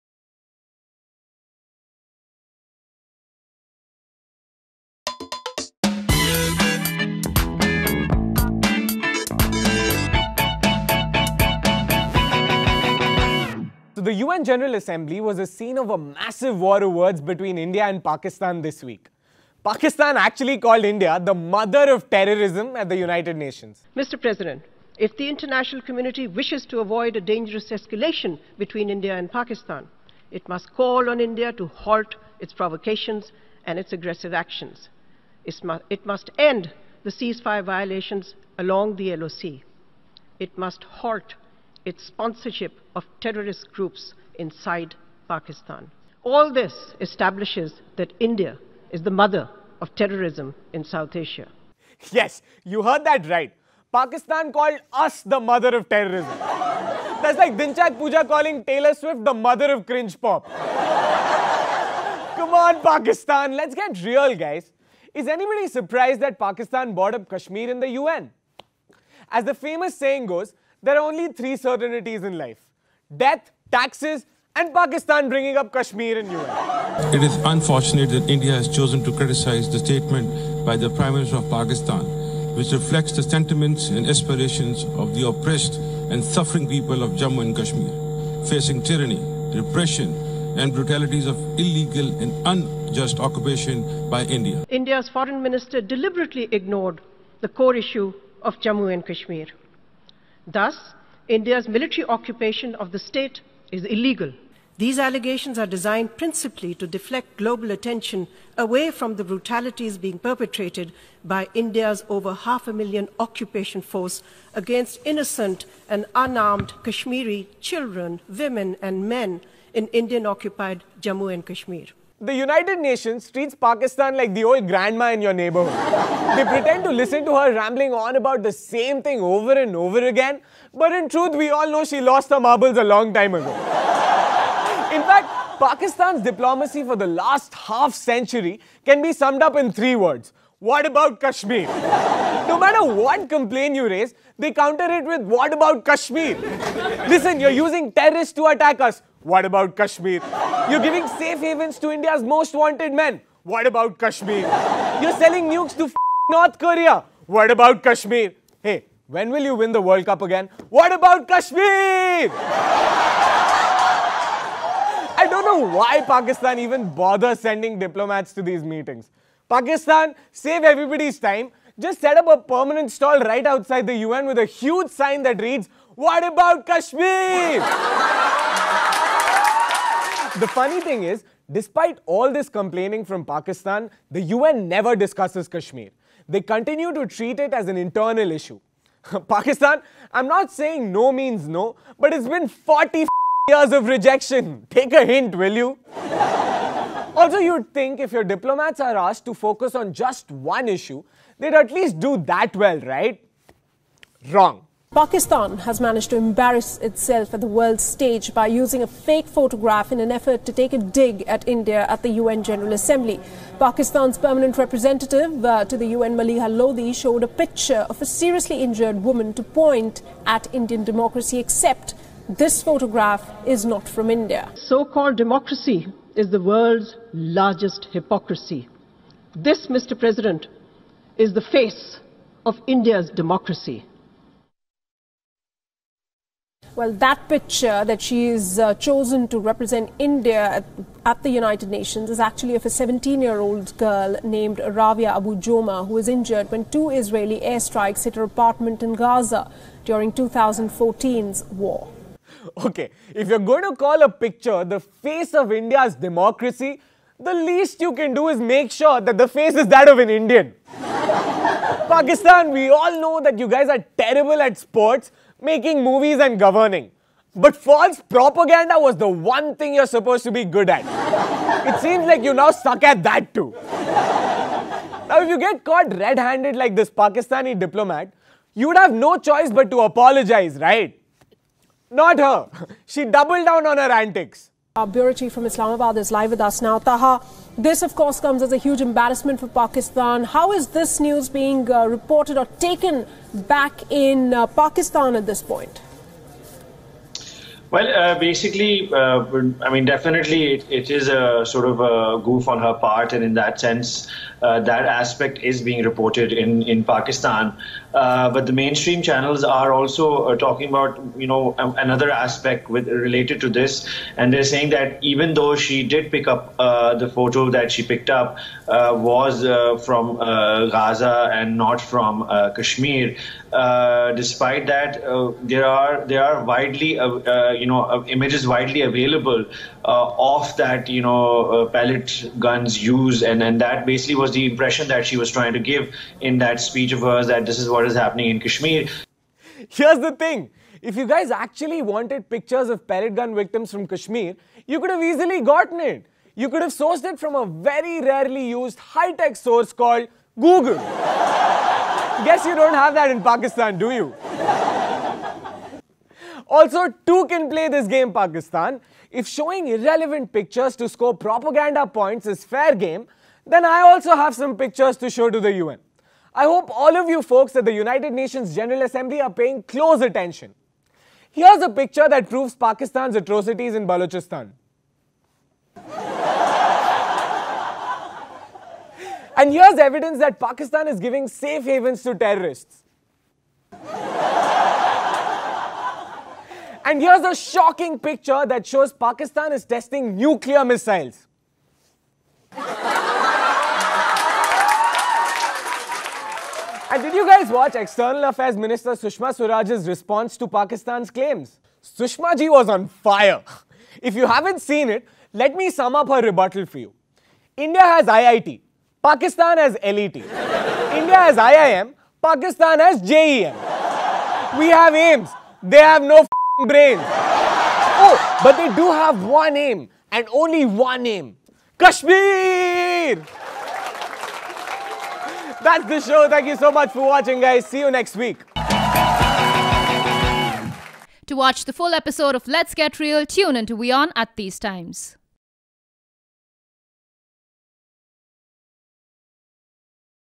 Hence Proved: Pakistan 'Mother of Stupid'
In the thirteenth episode of Let's Get Real, we give Pakistan a lesson on Google Search, talk about its stupid diplomacy and expose Pakistan through some exclusive photos of our own.